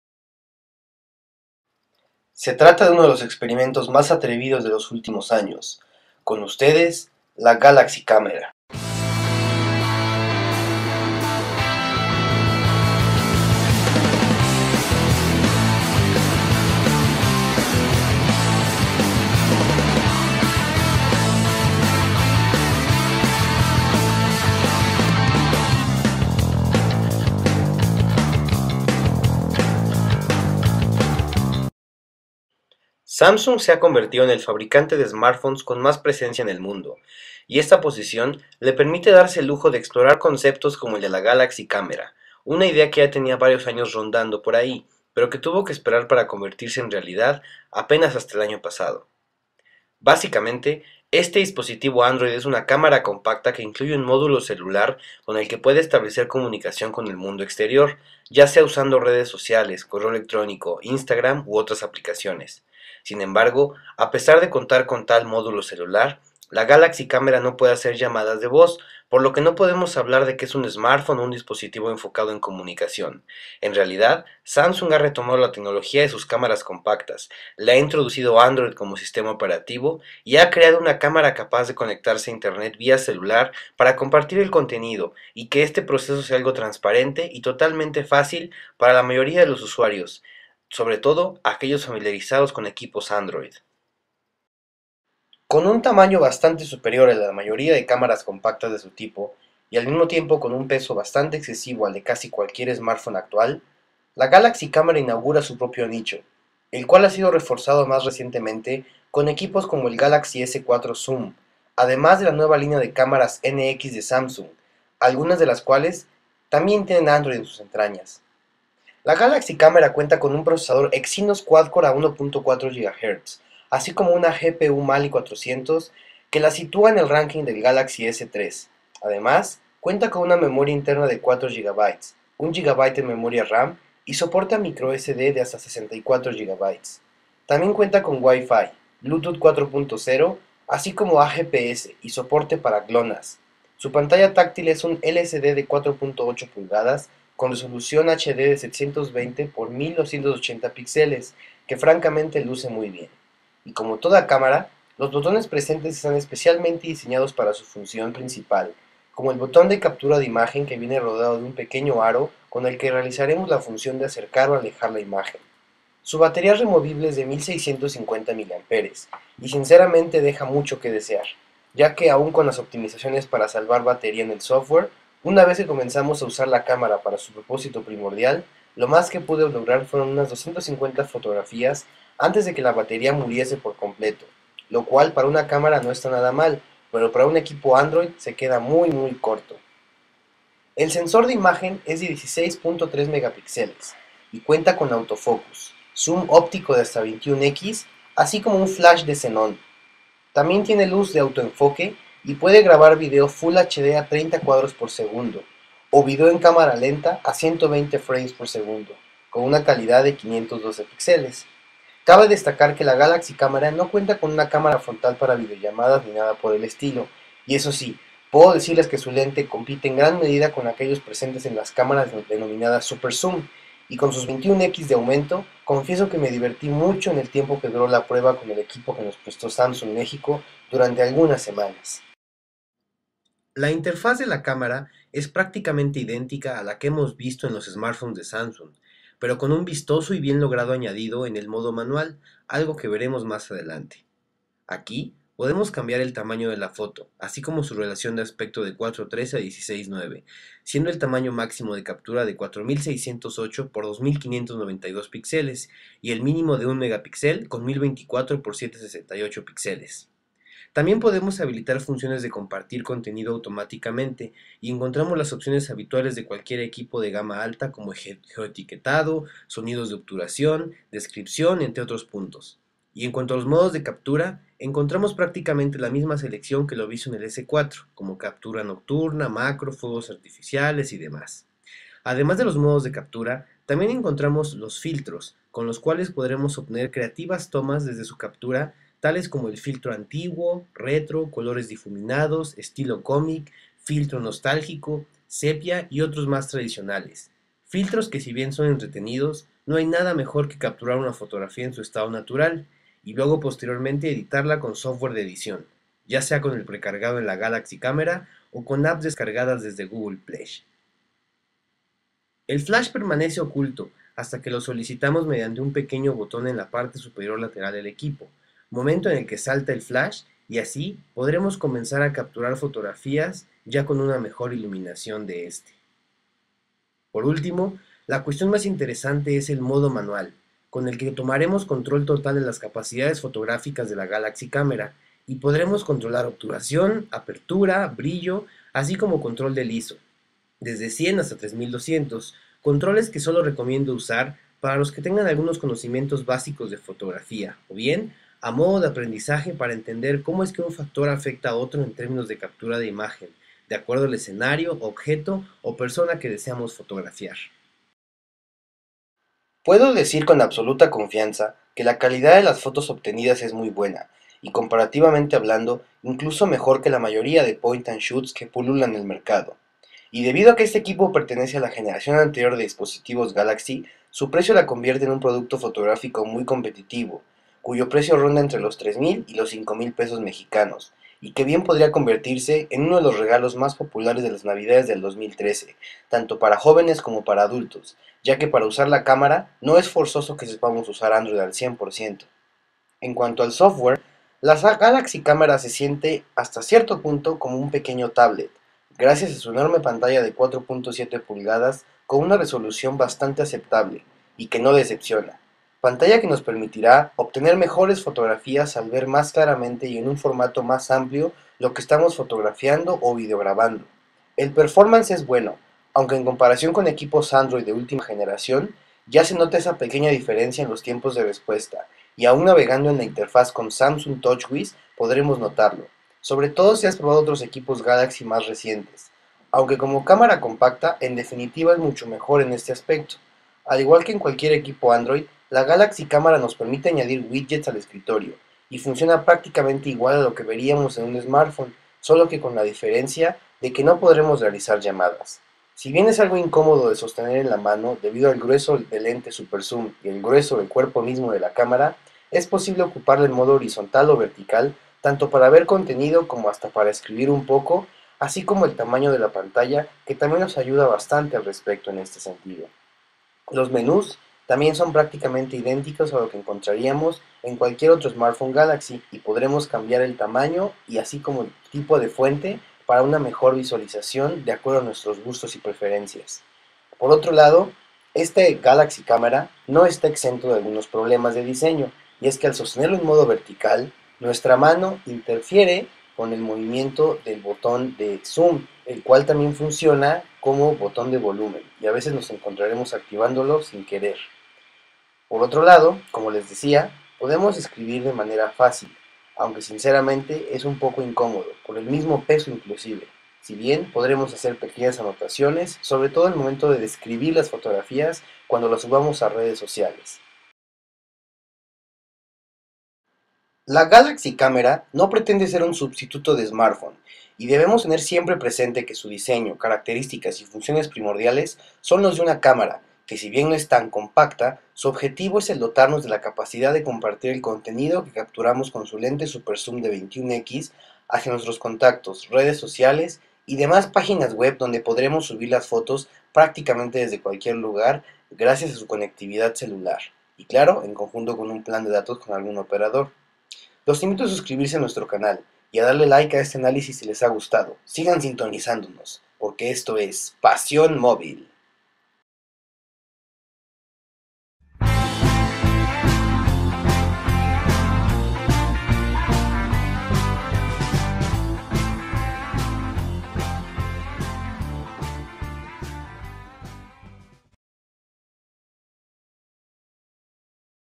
Samsung Galaxy Camera - Análisis en Español HD
Análisis en español Full HD de la Samsung Galaxy Camera, la primera cámara compacta de buena calidad con Sistema Operativo Android en sus entrañas. Conócela

PoderPDA - El blog sobre tecnología móvil